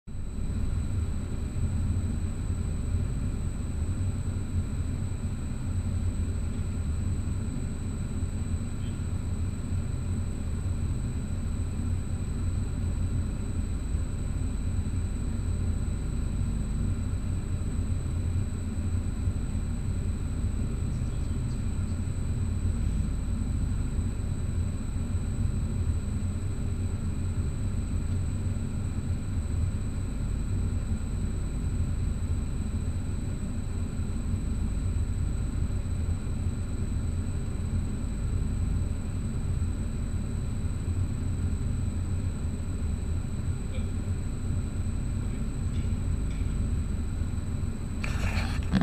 Flutter Exciter
Limit Cycle Oscillations at 12 m/s. Reynolds excited by Gurney flap moving at Flutter Frequency (2.2 Hz) ~ 6e4. ZHAW/SoE/ZAV Wind Tunnel.